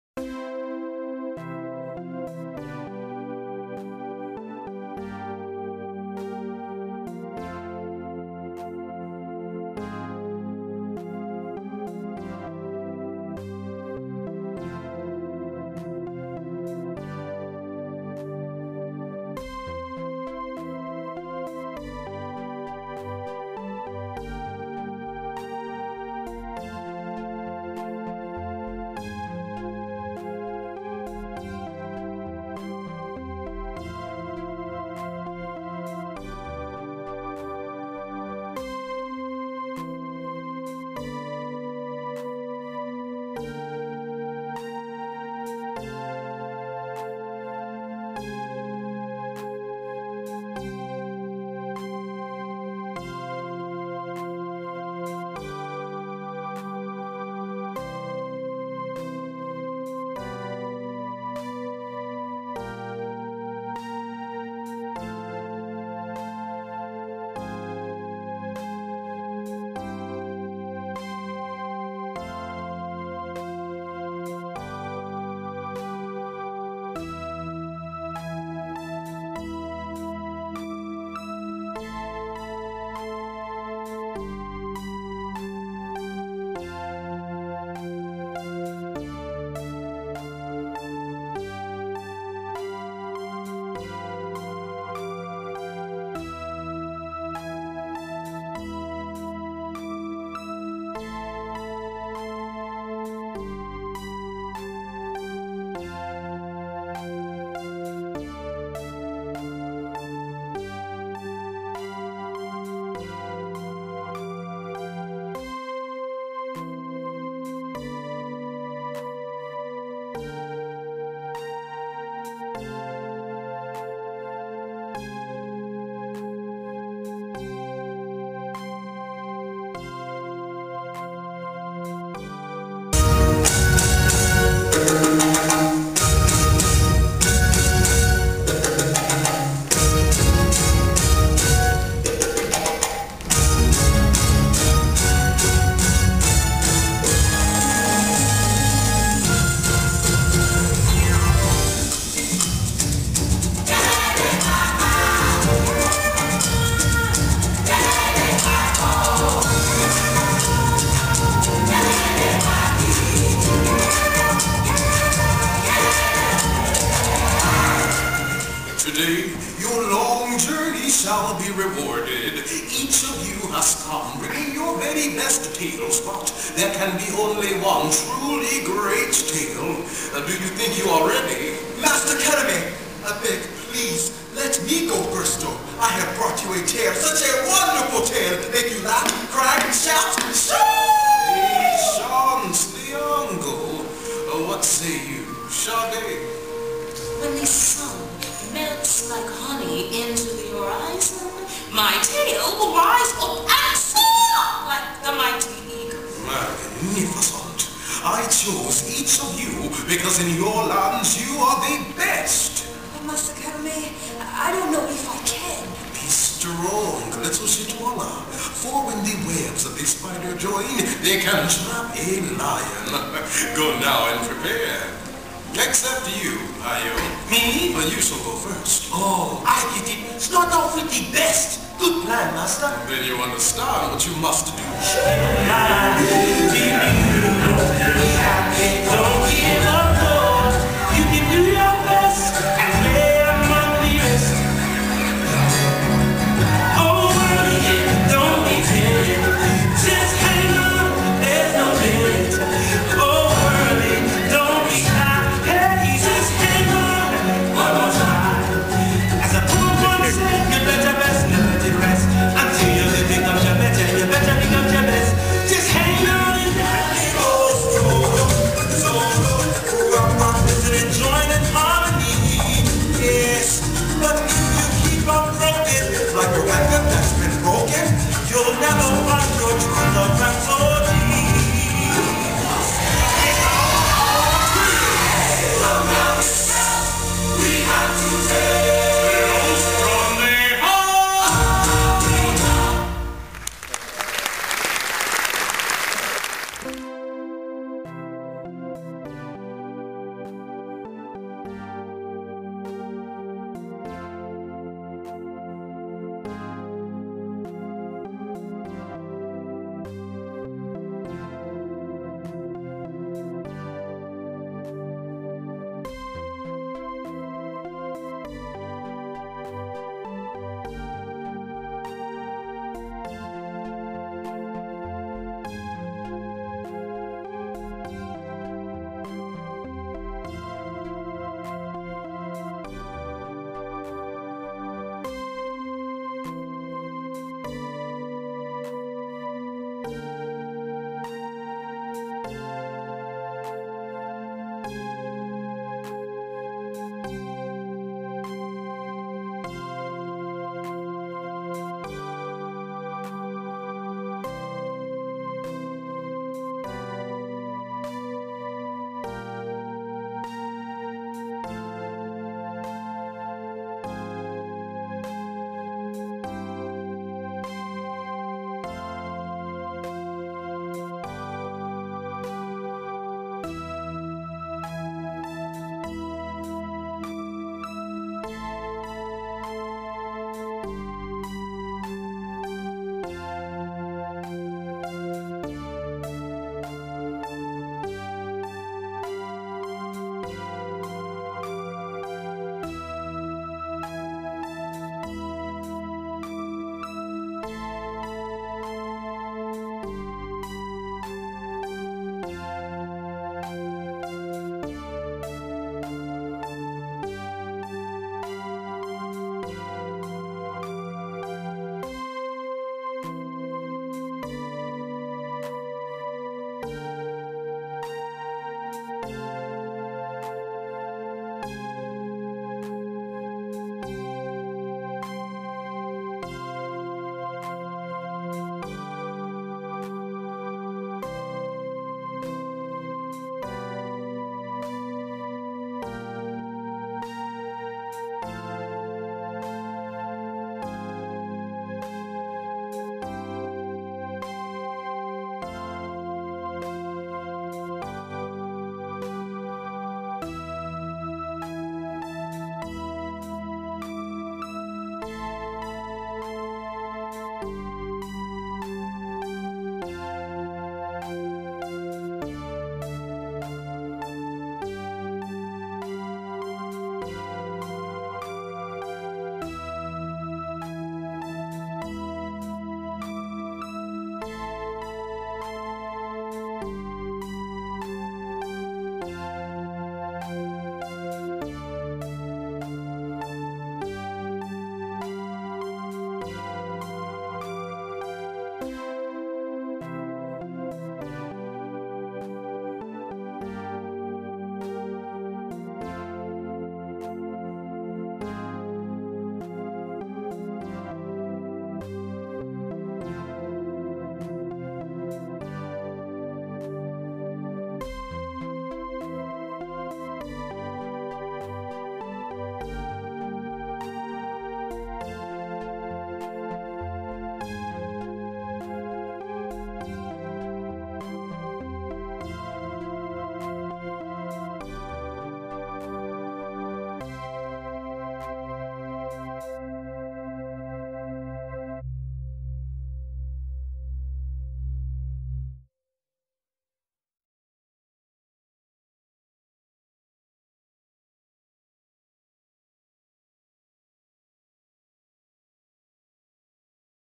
Bush Garden, Tampa, Florida 17 II 2009, part 1
W Tampie (Floryda) jest taki wielki park rozrywki,w  którym znajduje się zoo, miasteczko różnorakich zabaw dla dzieci (np. Roller Coster ;) ), czy ośrodek atrakcji artystycznych od muzycznych przez taneczne i aktorskie. Jest co podziwiać, co zwiedzać, a nawet gdzie pospacerować, nie wspominając o wyciągu krzesełkowym, którym można przejechać z kilku miejsc granicznych tego landu w inne.